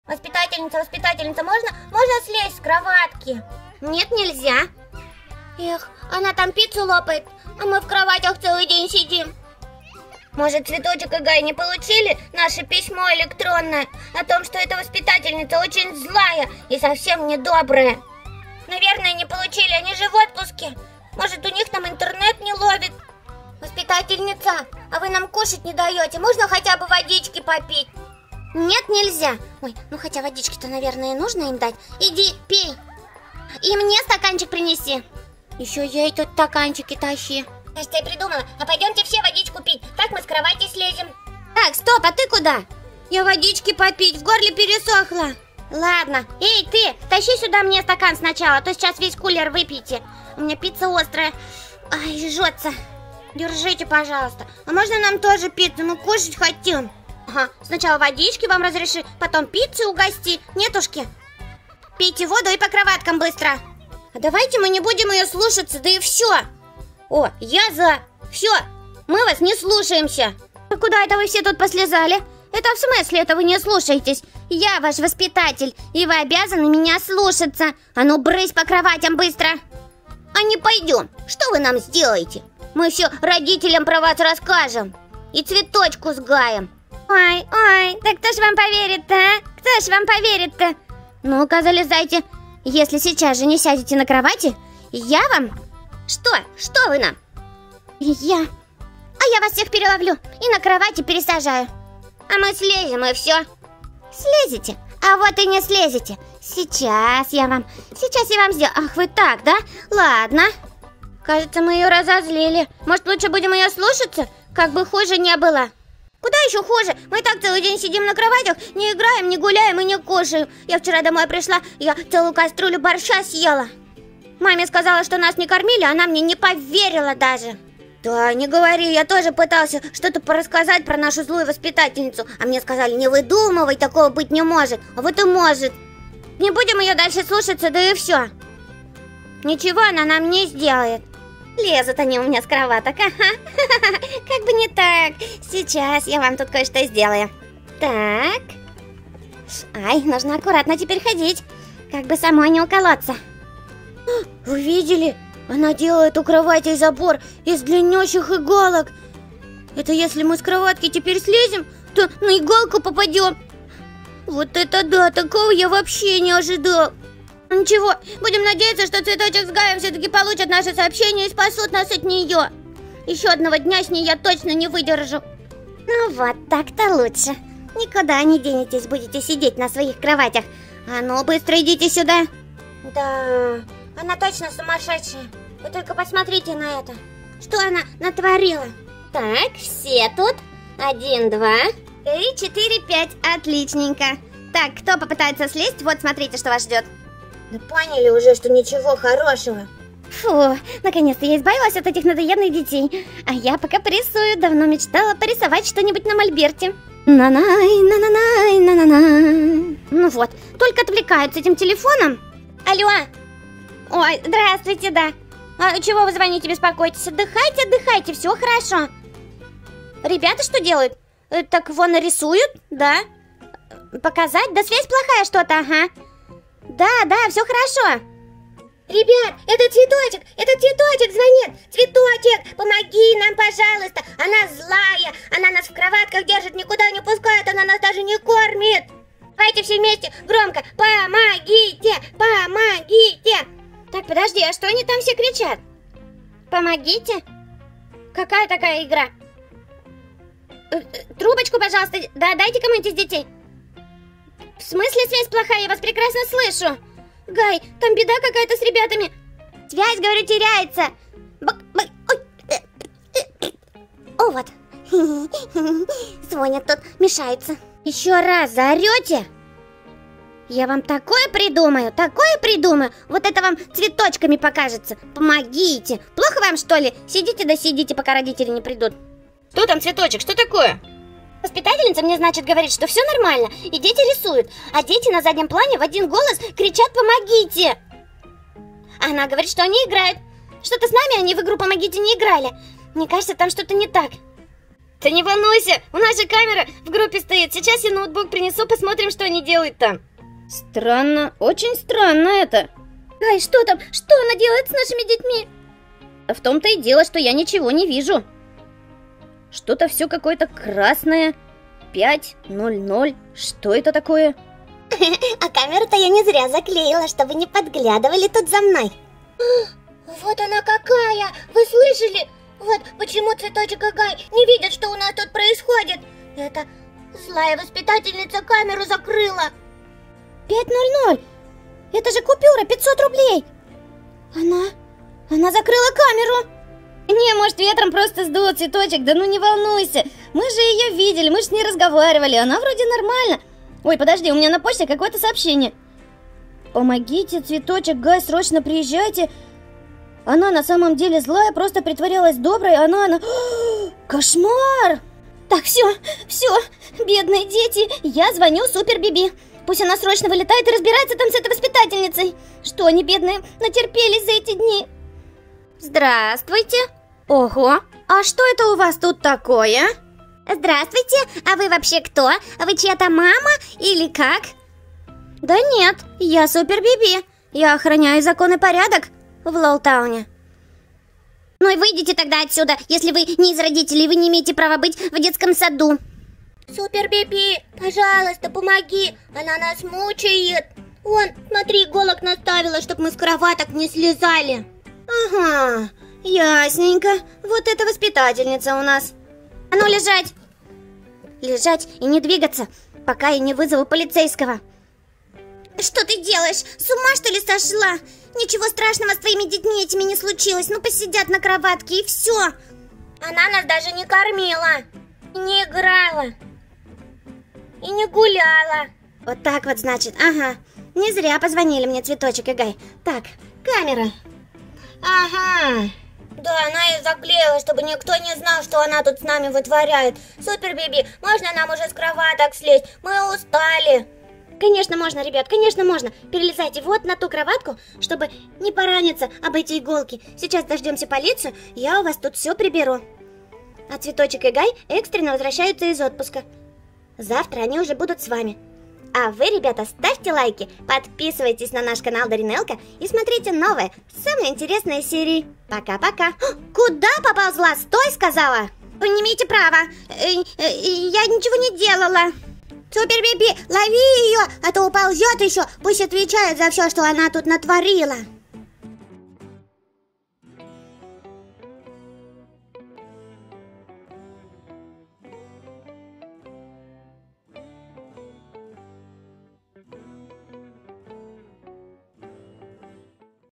ЗЛАЯ ВОСПИТАТЕЛЬНИЦА👿😱 НЕ ПУСКАЕТ ИГРАТЬ! Куклы ЛОЛ сюрприз в детском садике мультики ДАРИНЕЛКА
А так же заходи в нам в группу в Контакте